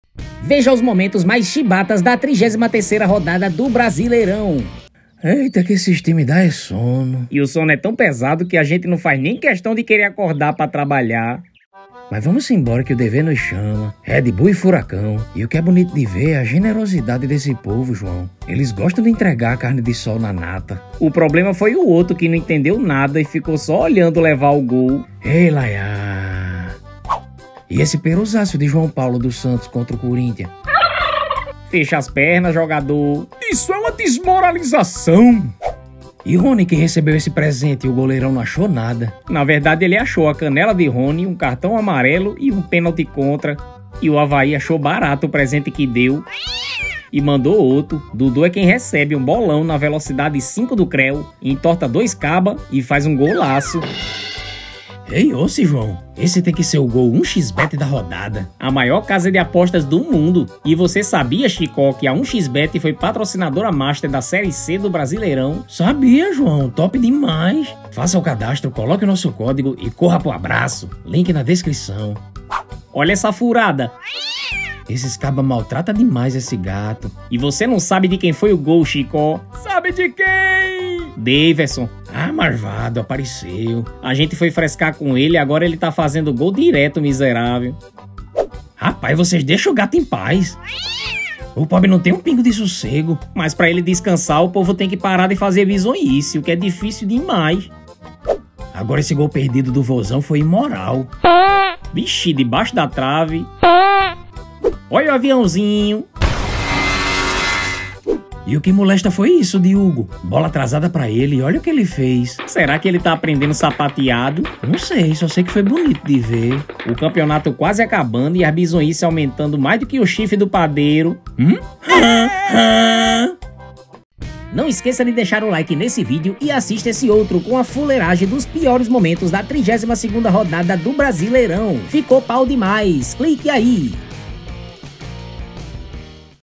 RUIM É APELIDO CARINHOSO! VEJA OS PIORES MOMENTOS DA 33ª RODADA DO BRASILEIRÃO 2022
ISSO É O SUMO DO FUTEBOL KKK. RUIM É APELIDO CARINHOSO! VEJA OS PIORES MOMENTOS DA 33ª RODADA DO BRASILEIRÃO 2022. Chicó e João tem que trabalhar muito haha. COMENTEM e COMPARTILHEM à vontade! Um abração, fiquem com Deus e valew!!!